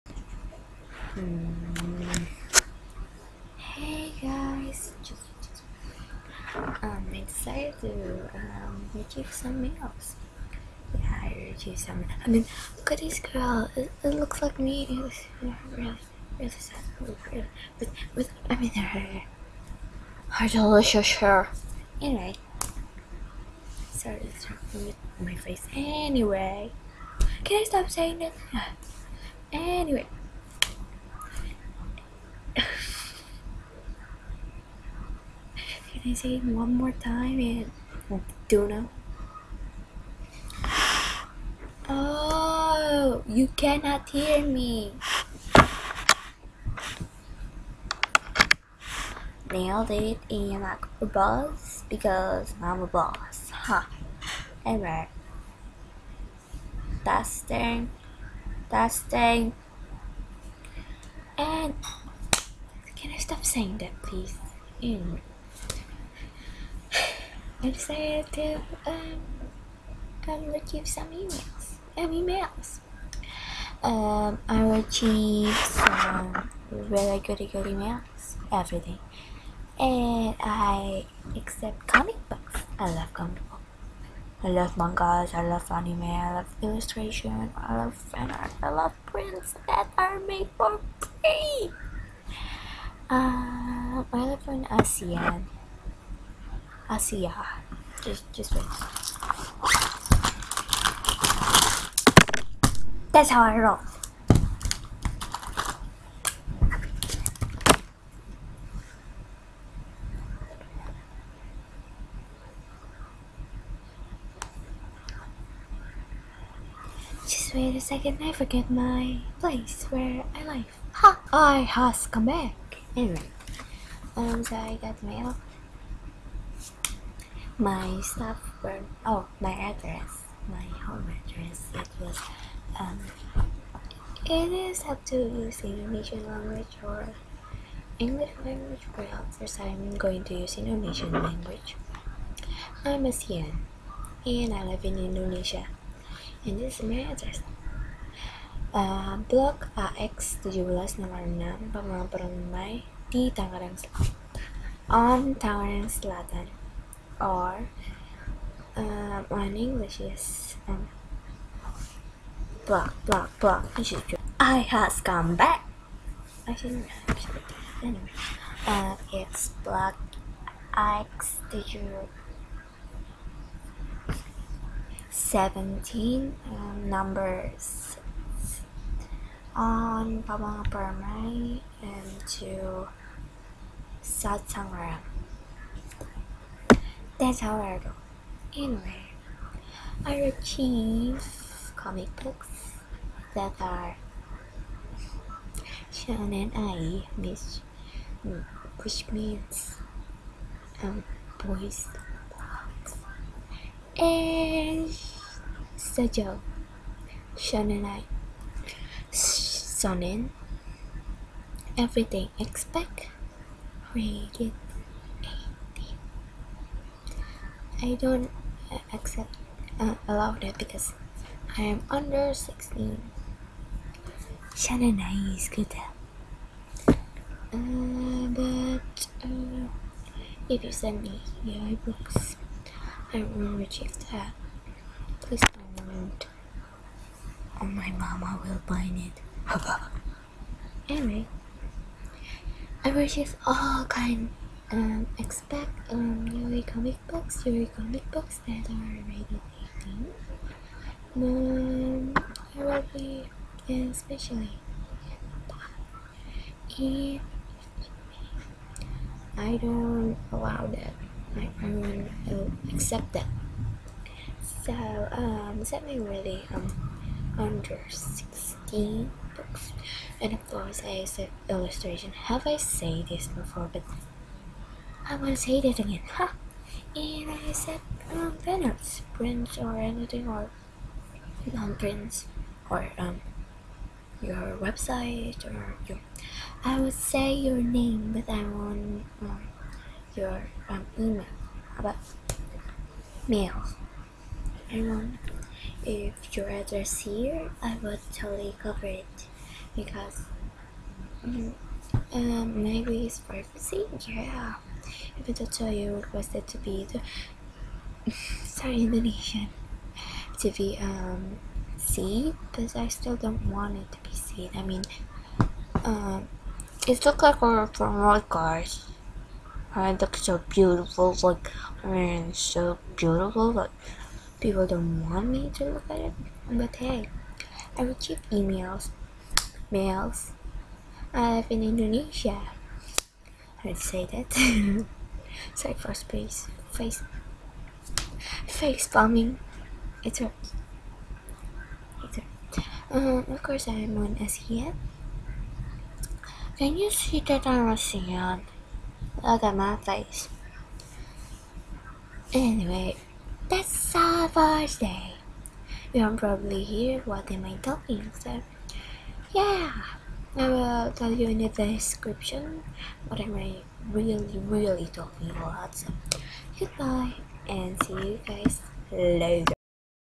IM RECEIVING MAILS! - MAILING TIME! (Ep1)
If You Live On Indonesia, Use JNE Express. I Dont Know How It Will Be Received On My Country, But I Think JNE Express Will Help. MY ADRESS: Blok Ax 17 No 6 ((Pamulang Permai On Tangerang Selatan)) Or In English Block Ax 17 Number 6 ((Pamulang Permai On Tangerang South.))